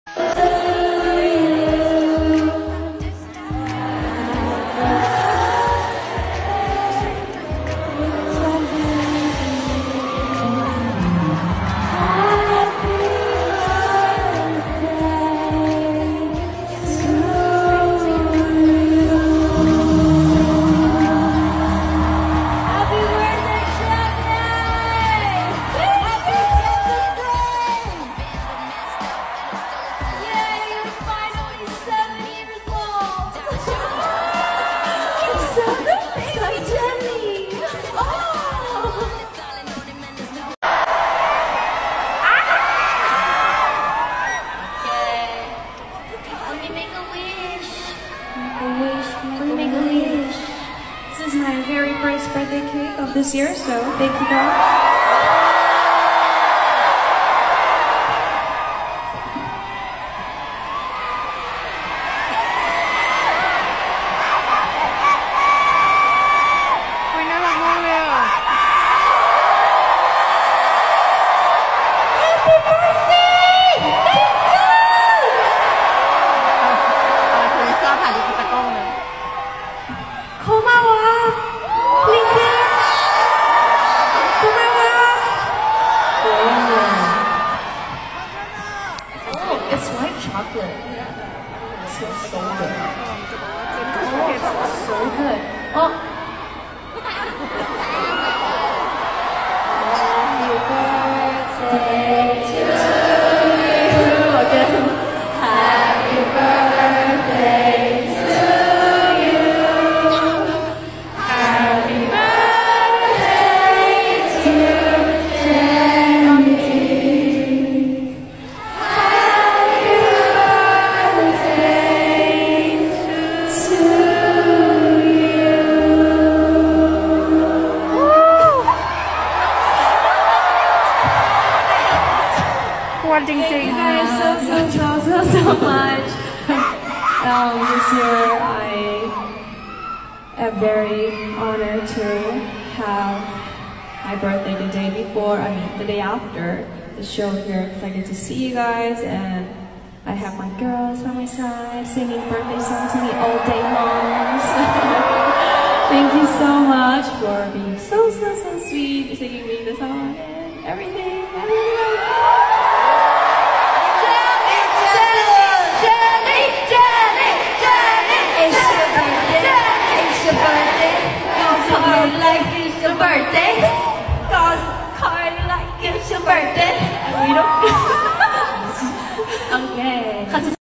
Blackpink 블랙핑크 celebrates Jisoo's birthday at a concert tour in Hongkong
blackpink